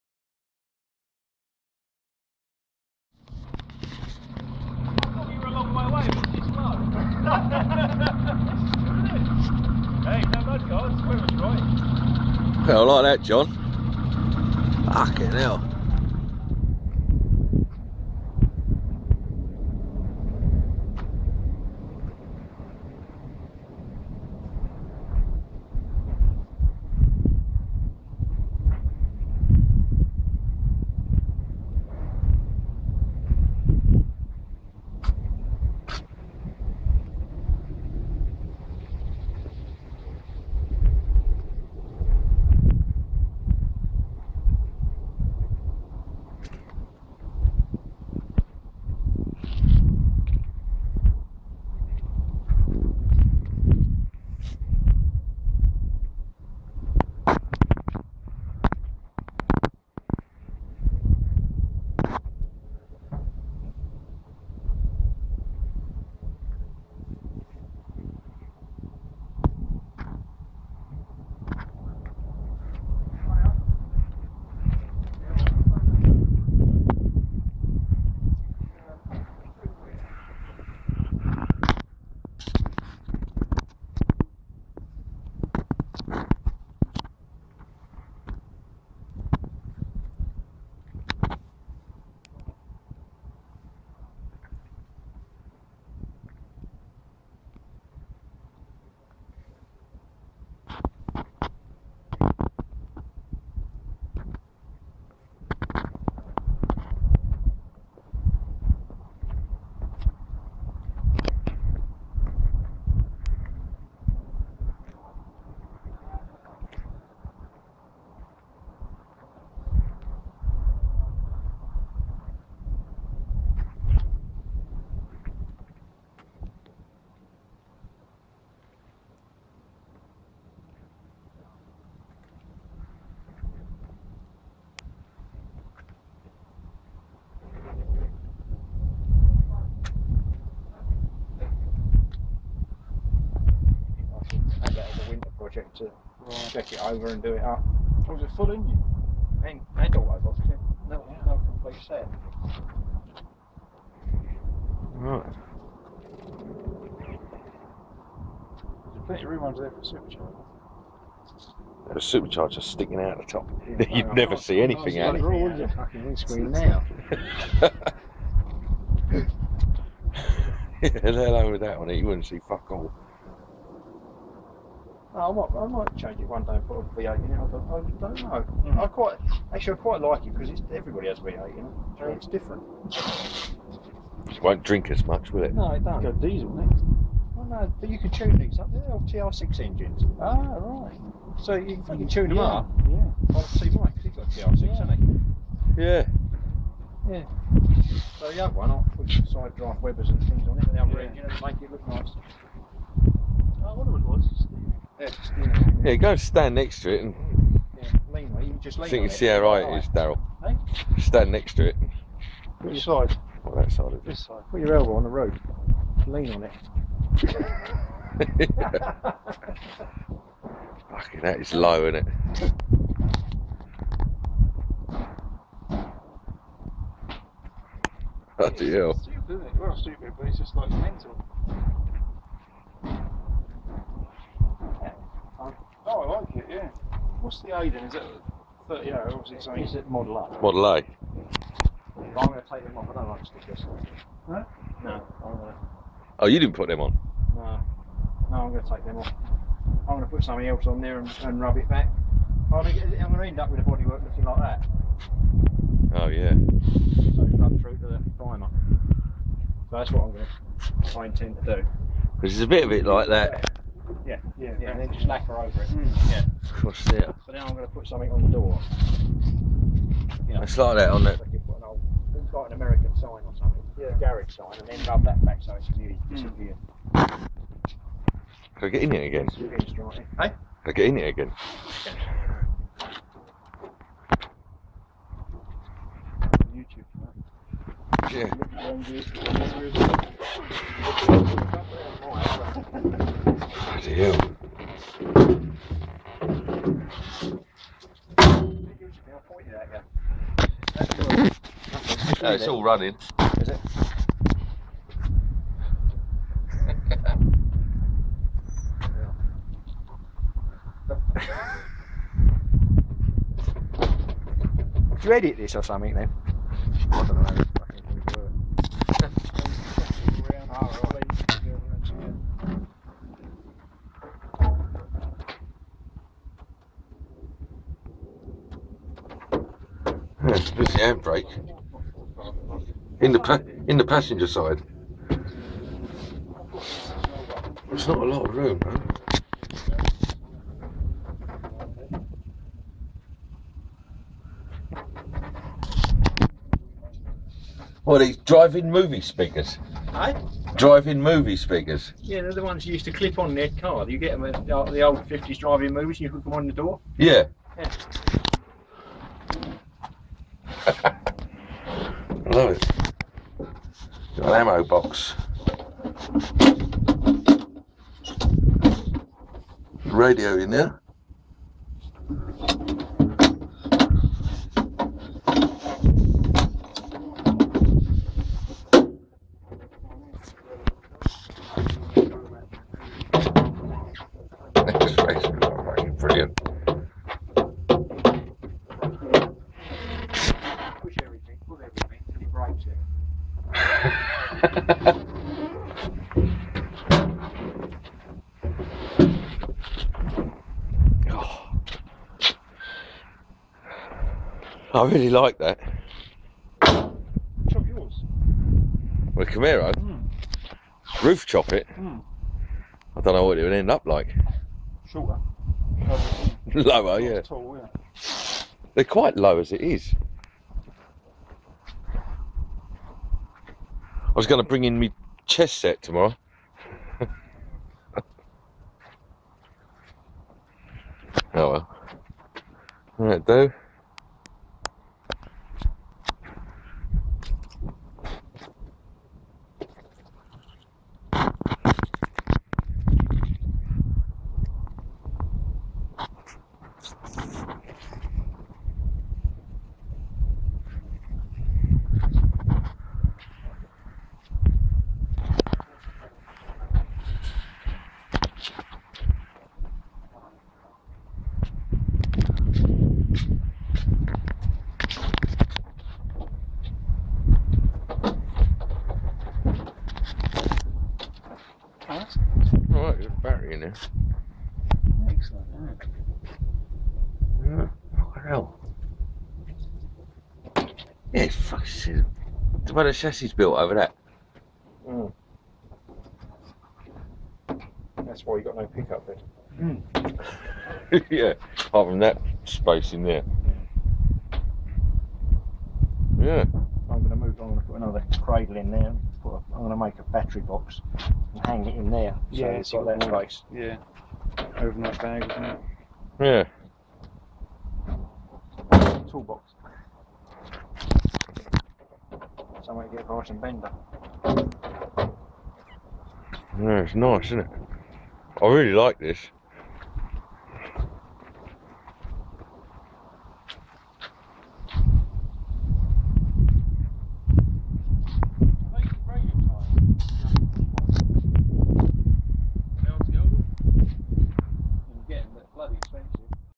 1930 Ford Model A pickup, RAT ROD, Roof chopped, slammed.
Guy at work brought in his model A pickup. Amazing looking!